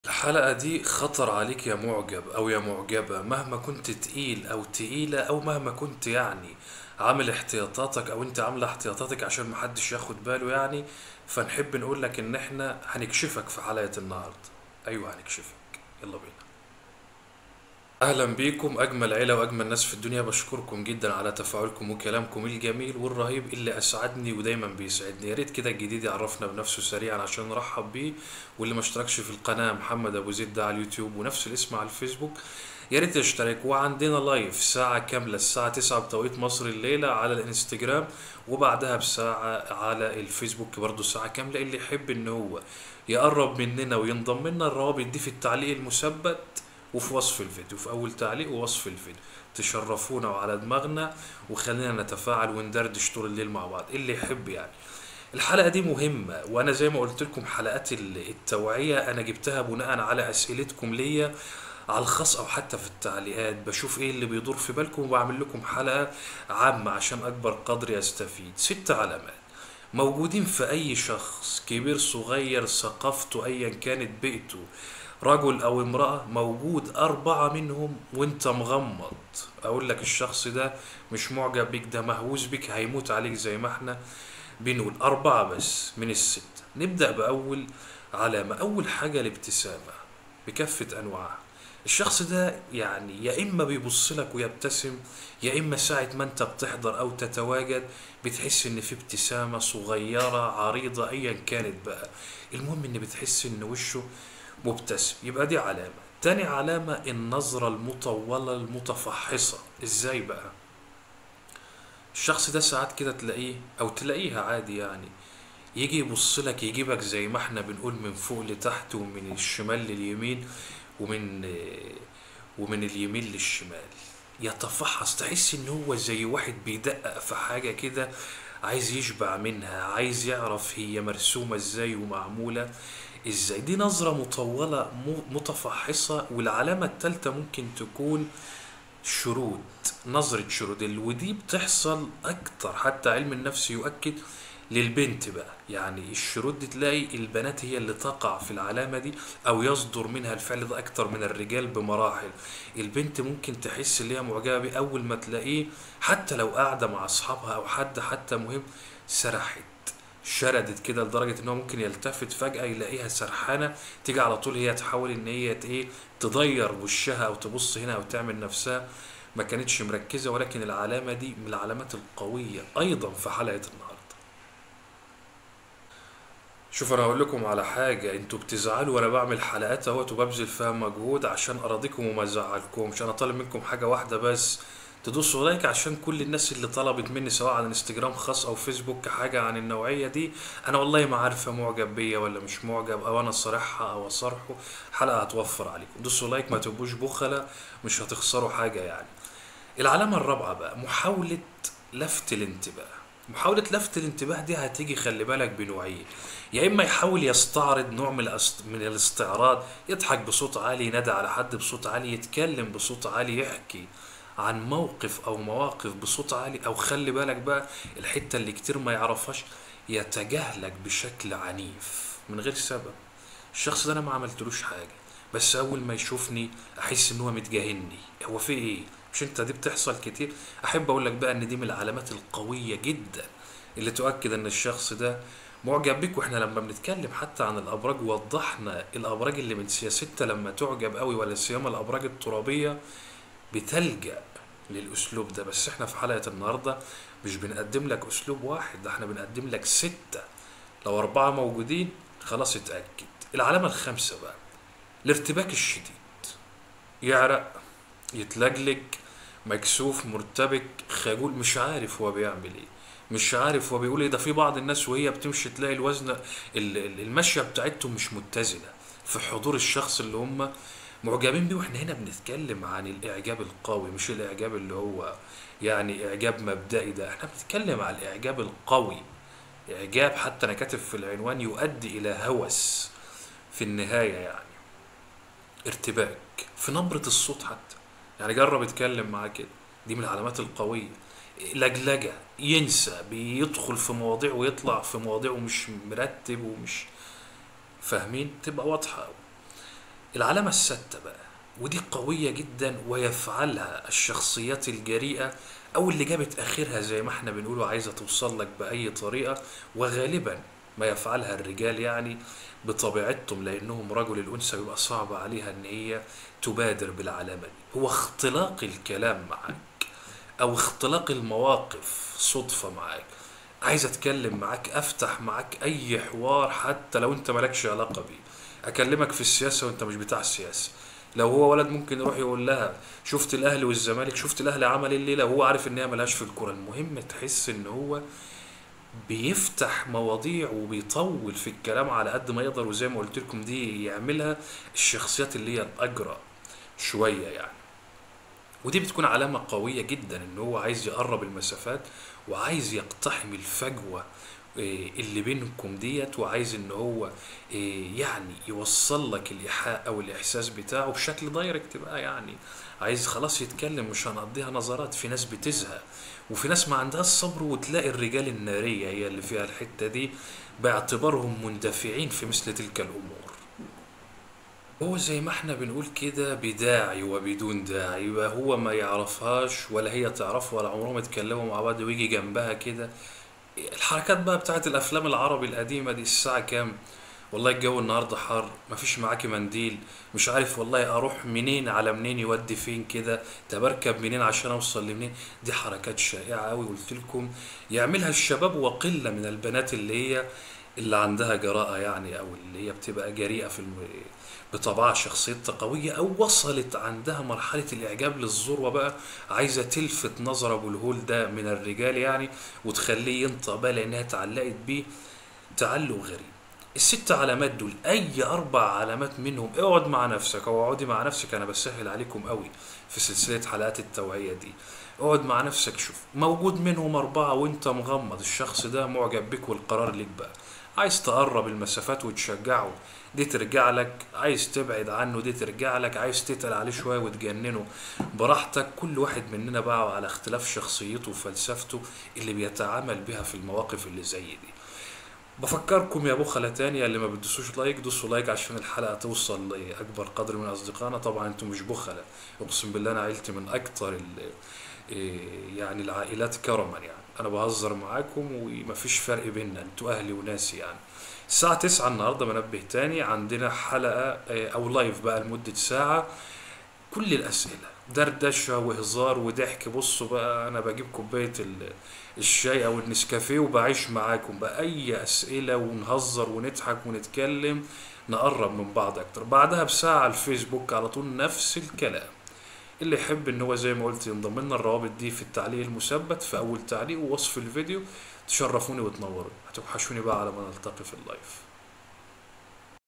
ستة علامات تفضح المعجب او المعجبة اذا وجدت اربعة منها فهو مهووس بك😍|محمد أبوزيد
اتمنى من اى حد عندو صفحة فيسبوك يشترك ف صفحتنا علشان يشوف فيديوهتنا ويحضر الايف معانا